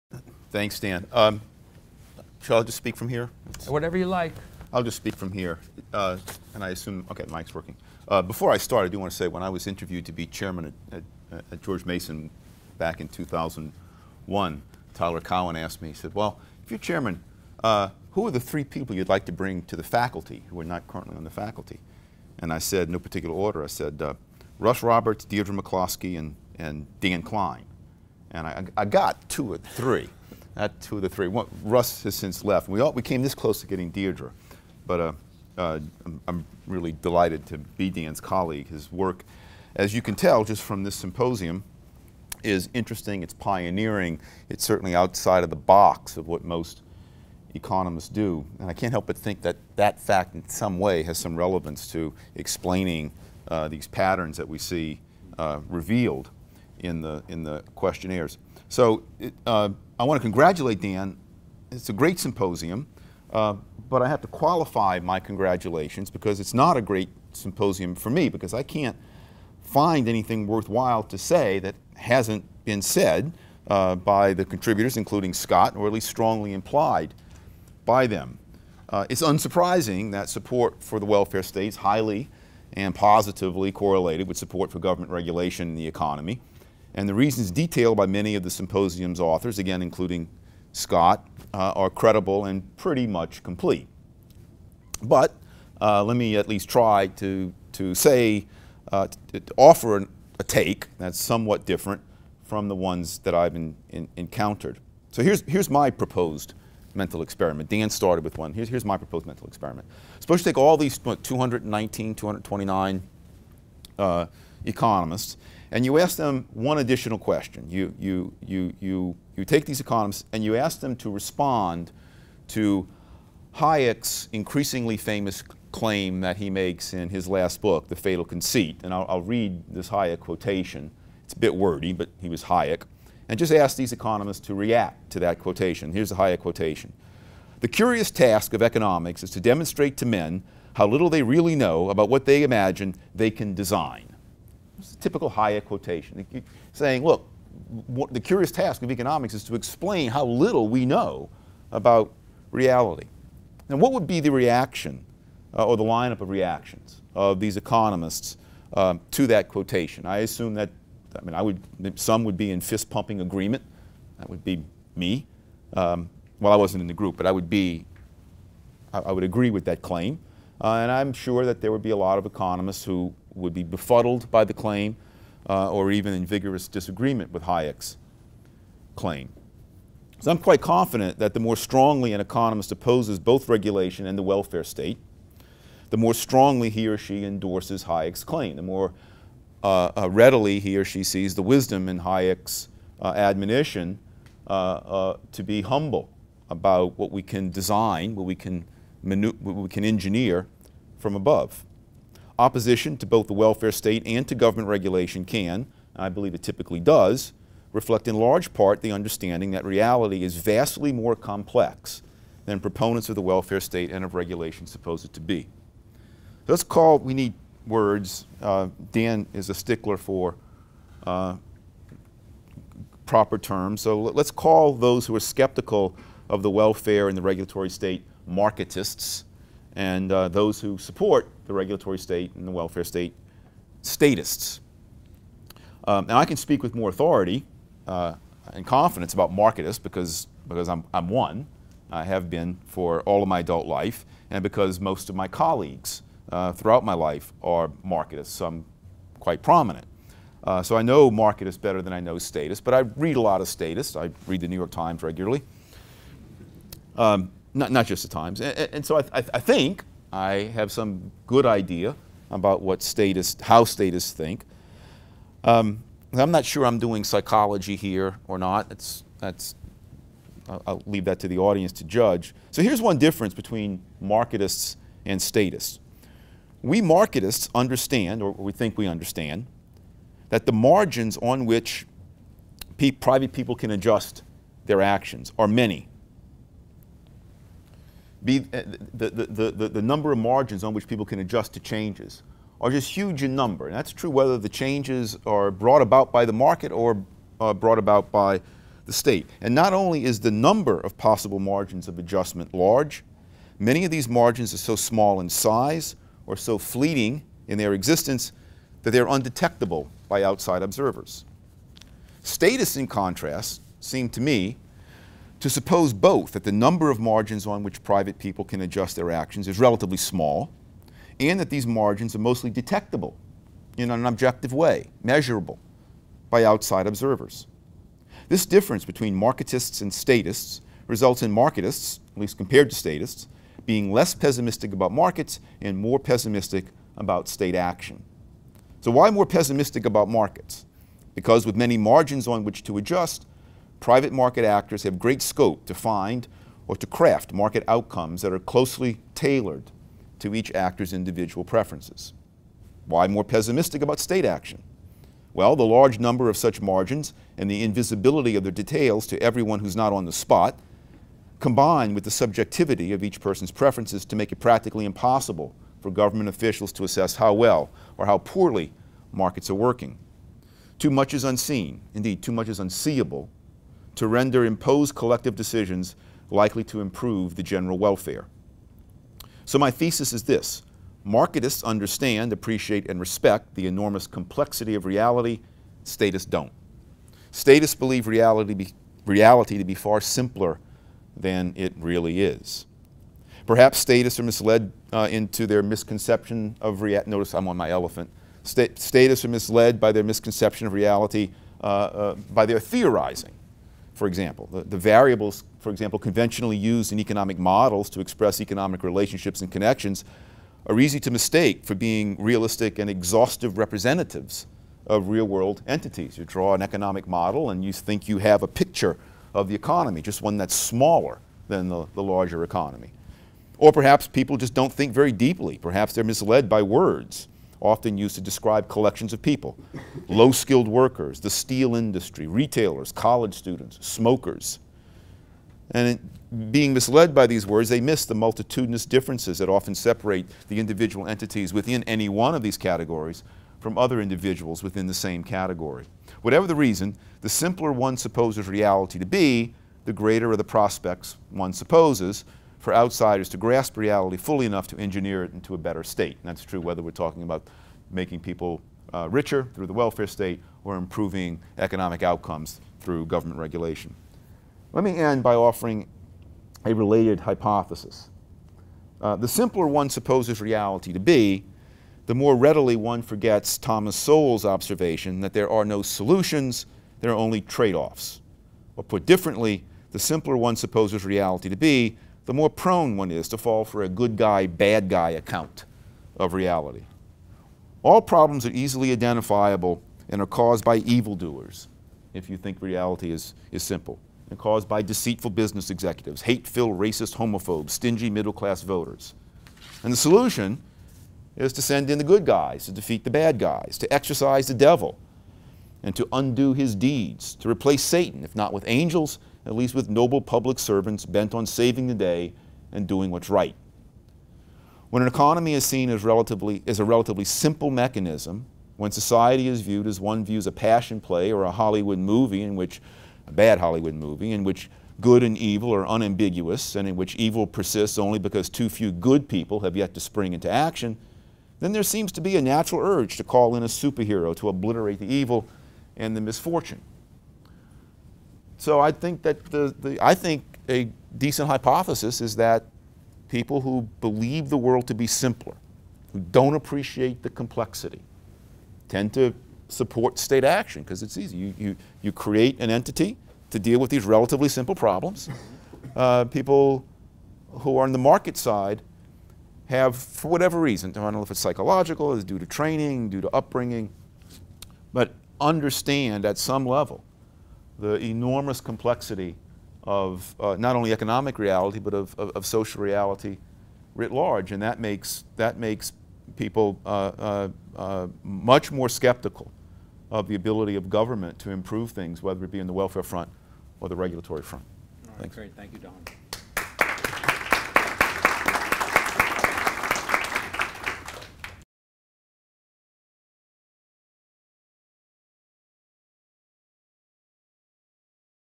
Don Boudreaux | Economists on the Welfare and Regulatory States | EJW Panel
The welfare state and the regulatory state are two policy areas that rarely elicit converging positions, but is it possible to find an economist who defies this pattern? In the January 2015 issue of Econ Journal Watch, Chief Editor Daniel Klein has prompted contributors and scholars to consider why so few economists argue in favor of one and against the other, and whether they can think of some economists who have done so.

Daniel Klein is Professor of Economics and JIN Chair at the Mercatus Center at George Mason University. He is also the chief editor of Econ Journal Watch and the leader of the Adam Smith Program among others at George Mason University. He has published research on a number of issues including spontaneous order, credit reporting, the FDA, government intervention and policy, and the relationship between liberty, dignity, and responsibility.

Donald Boudreaux is Professor of Economics, Martha and Nelson Getchell Chair for the Study of Free Market Capitalism, and Senior Fellow with the F.A. Hayek Program for Advanced Study in Philosophy, Politics, and Economics at the Mercatus Center at George Mason University. He specializes in globalization, trade, law and economics, and antitrust economics. and has recently published a book with the Fraser Institute titled The Essential Hayek.

Scott Sumner is Professor of Economics at Bentley University and the Ralph G. Hawtrey Chair and Program Director of Monetary Policy at the Mercatus Center at George Mason University. He specializes in monetary policy, the role of the international gold market in the Great Depression, and the history of macroeconomic thought. he has been published in journals such as the Journal of Political Economy, Economic Inquiry, and the Journal of Money, Credit and Banking.

Jeremy Rabkin is Professor of Law at George Mason University. He is also currently on the Board of Directors for the United States Institute of Peace and the Center for Individual Rights, and the Board of Academic Advisors for the Harvard Journal of Law and Public Policy and American Enterprise Institute. Prior to joining George Mason University, he was a professor in the Department of Government at Cornell University from 1980 – 2007 and a visiting professor in the Department of Government at Harvard University in 1993.